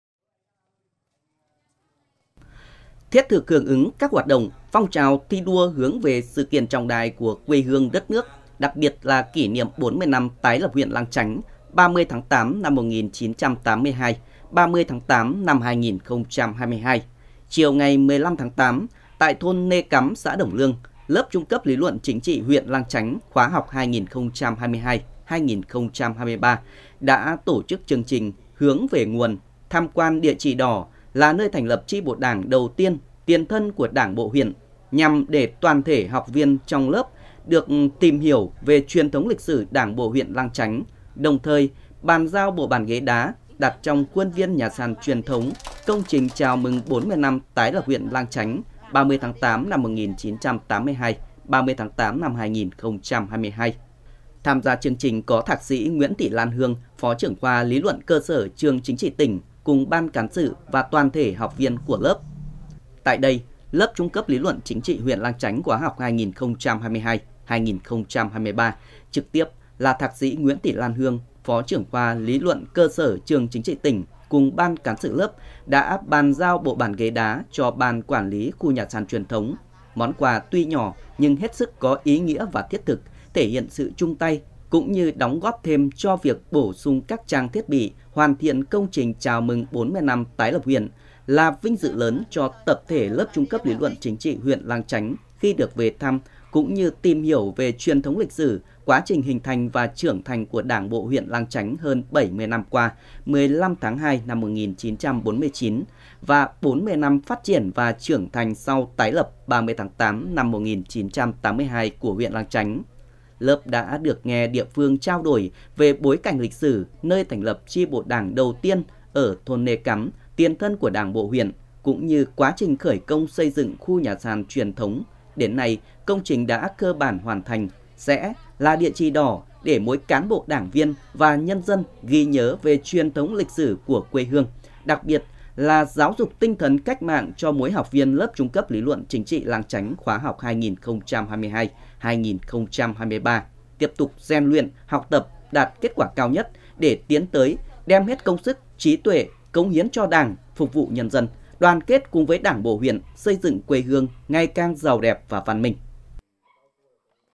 Lớp Trung cấp LLCT huyện, hướng về nguồn nơi thành lập Chi bộ Đảng, tiền thân của Đảng bộ huyện
Lớp Trung cấp LLCT huyện Lang Chánh khóa 2022 – 2023, hướng về nguồn nơi thành lập Chi bộ Đảng, tiền thân của Đảng bộ huyện